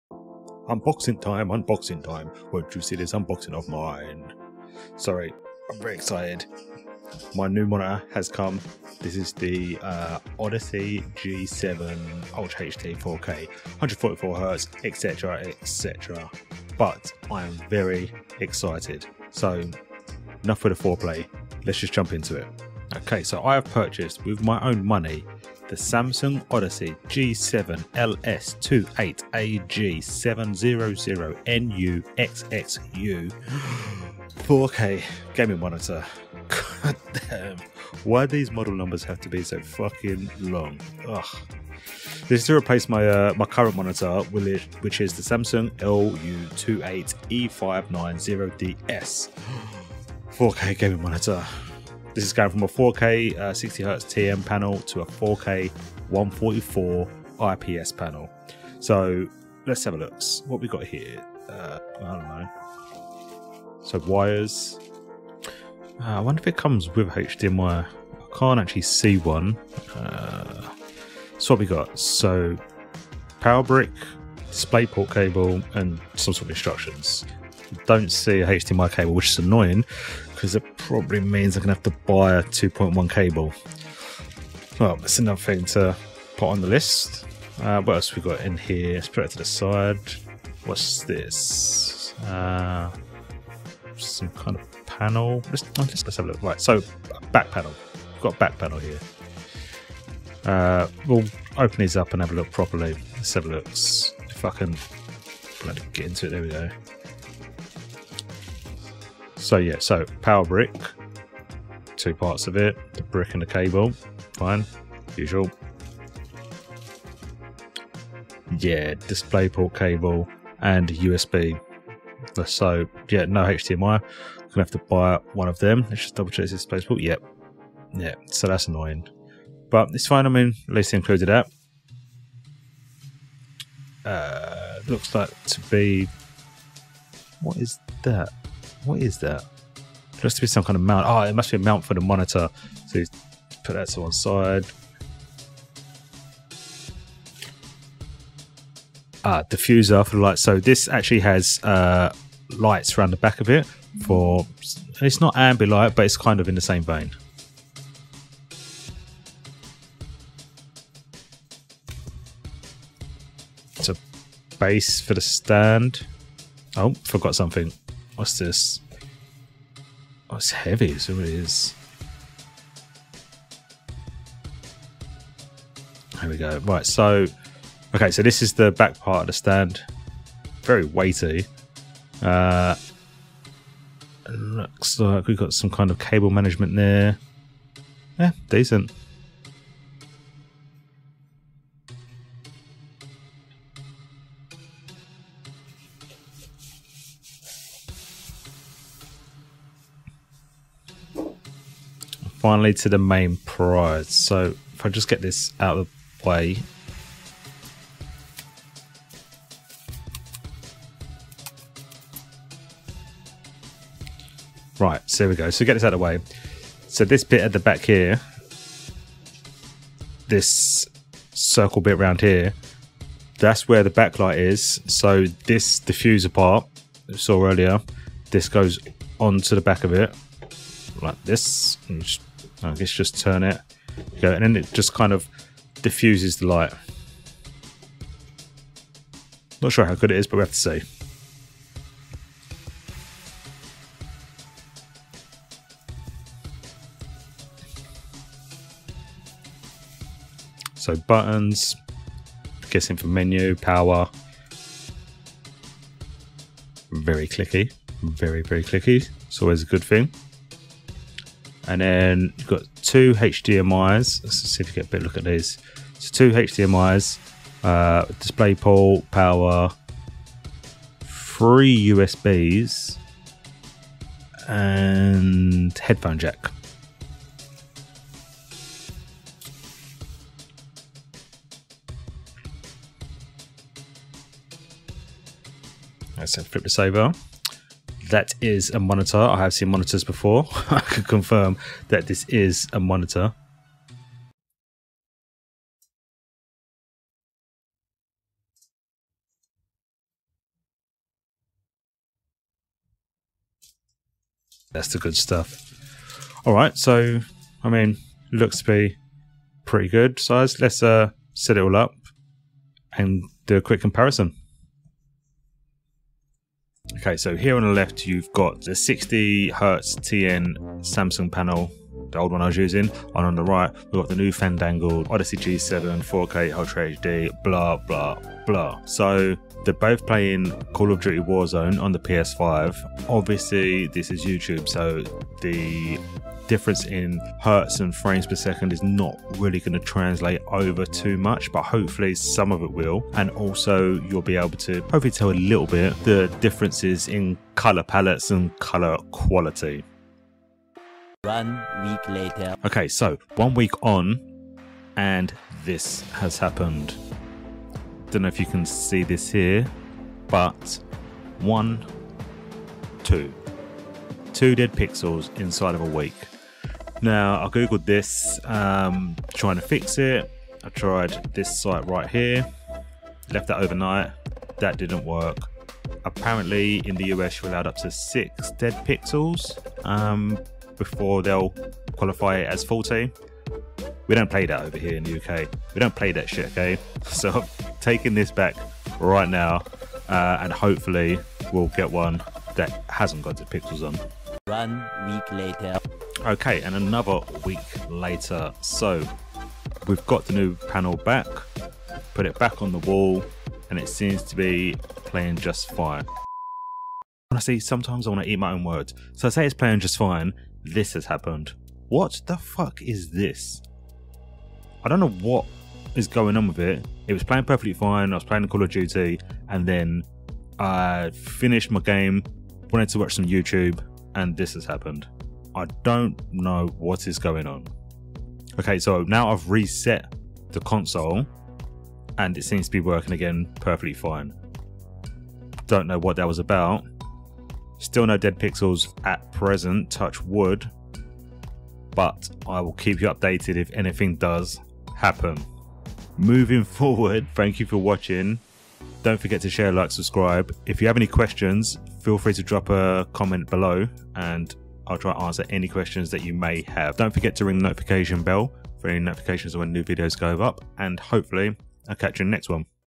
*NEW* Samsung Odyssey G70A 4K Gaming monitor - Unboxing
Finally took the plunge and opted for the new Samsung Odyssey G7 4K gaming monitor, although it has not been without its mishaps and problems come unbox with me